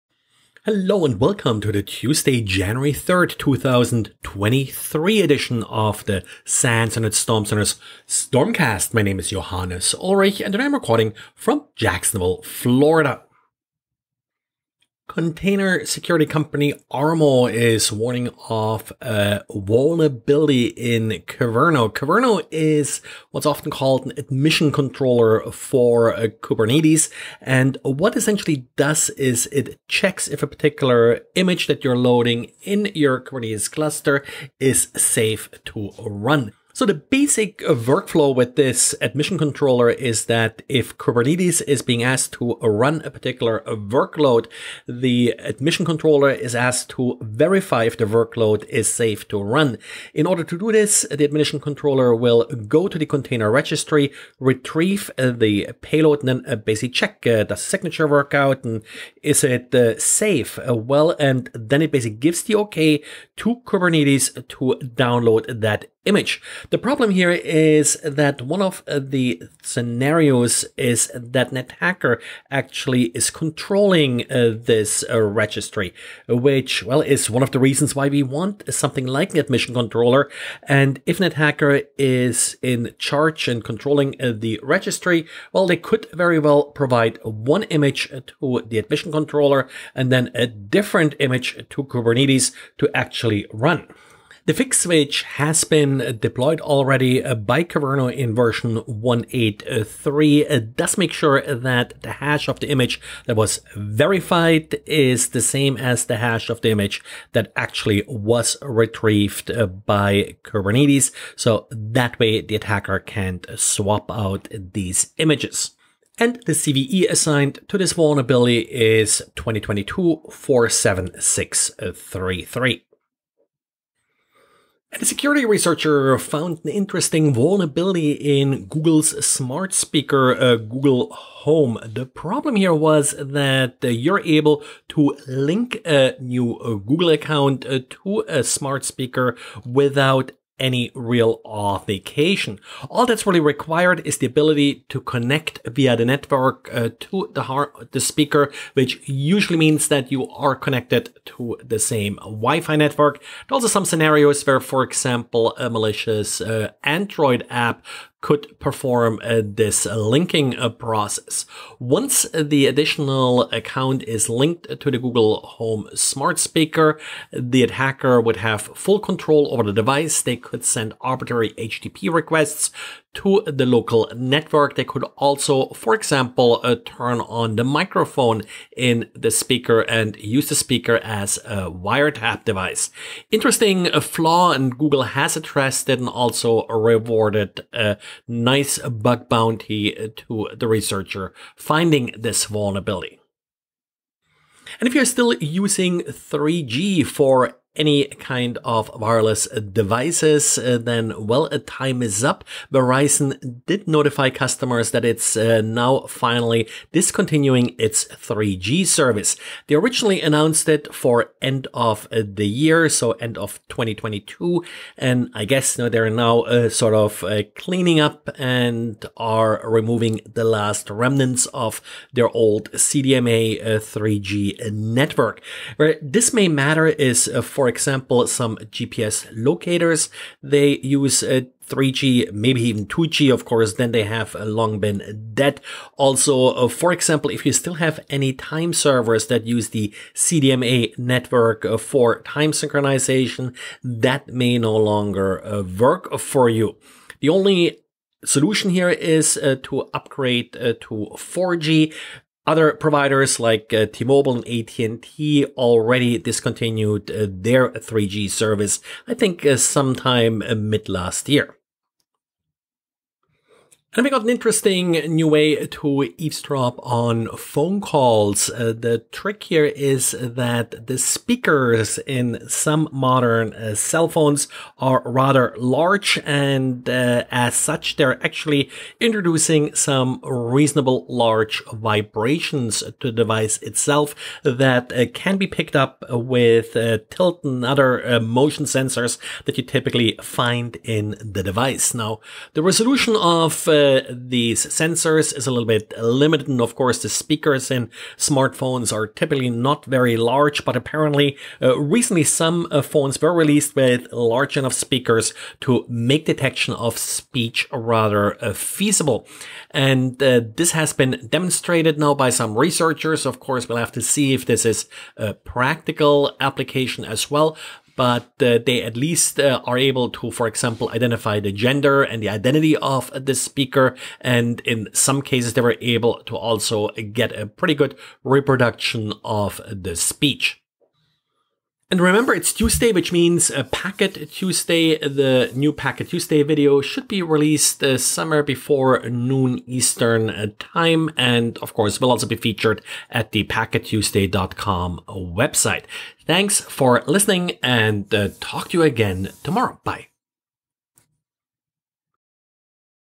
Network Security News Summary for Tuesday January 3rd, 2023
Kyverno image swap vuln; Google Home Vuln; 3G CDMA Decomissioning; EarSpy Cell Phone Evesdropping

EarSpy: Spying Caller Speech and Identity Through Speaker Vibrations